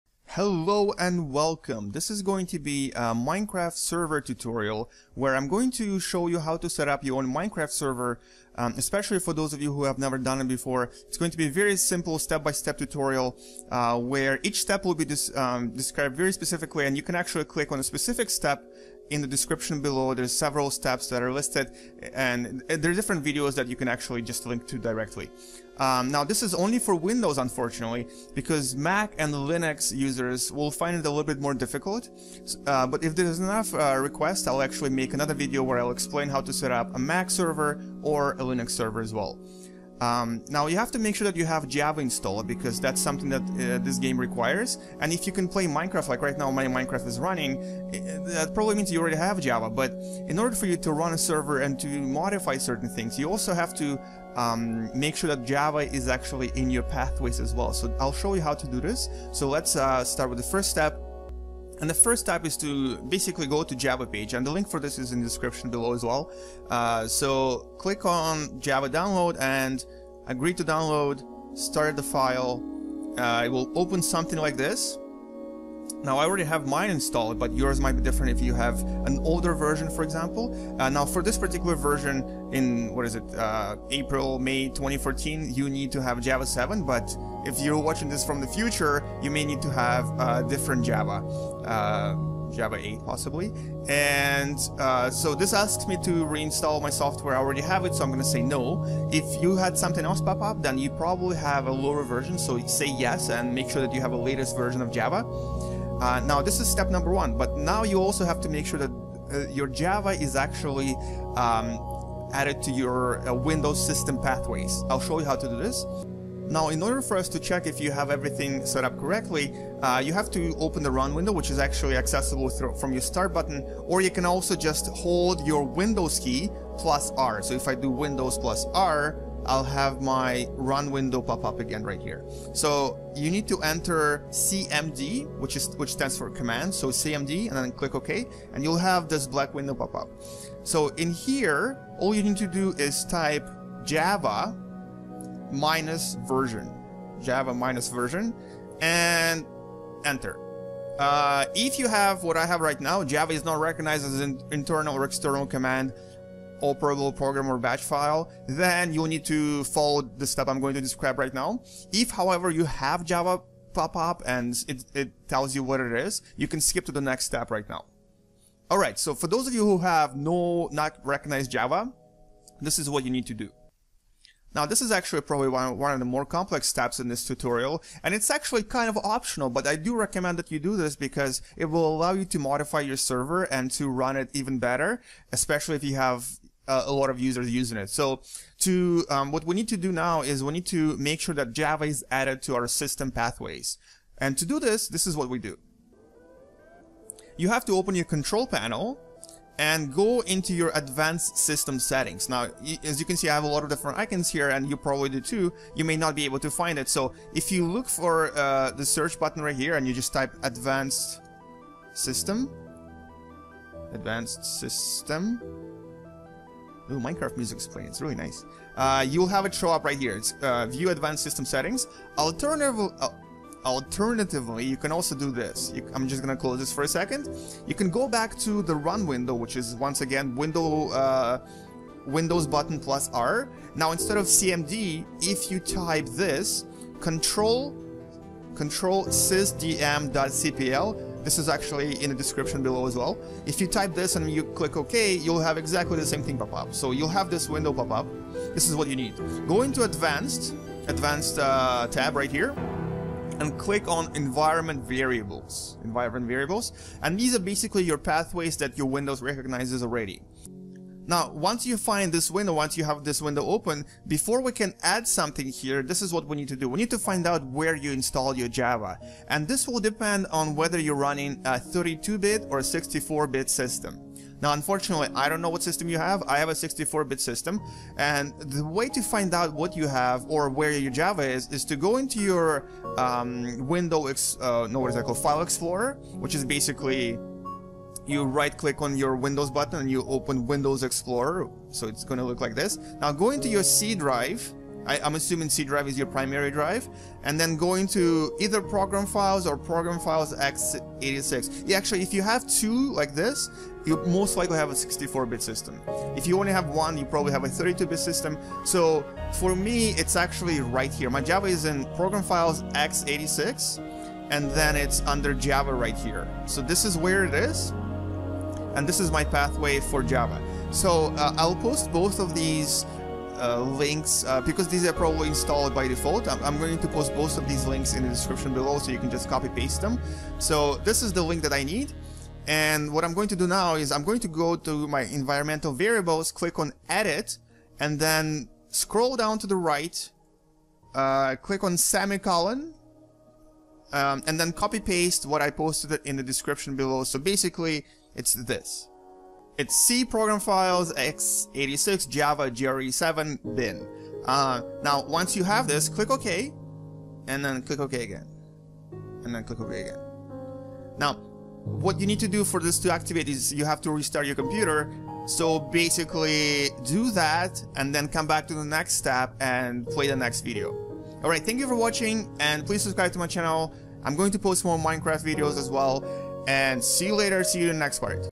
Java Installation and Setup - Minecraft SERVER tutorial 1/4
Hello and welcome to this tutorial on how to set up and run your own Minecraft server.

This is a step by step and easy to follow tutorial that explains everything in easy to comprehend words.

This first part is on how to setup your JAVA that you need to run the server and how to make sure that you can modify your server settings later.

To check your JAVA version, go into Command Prompt (click on start button, go to RUN and type CMD then enter) and then type

java --version

To open the advanced system window type

control sysdm.cpl

What you need to enter under pathways is this.

For 32-bit systems:
;"c:\Program Files\Java\jre7\bin"

For 64-bit systems:
;"c:\Program Files (x86)\Java\jre7\bin"

Make sure that there's only one semicolon between your previous entry and this entry.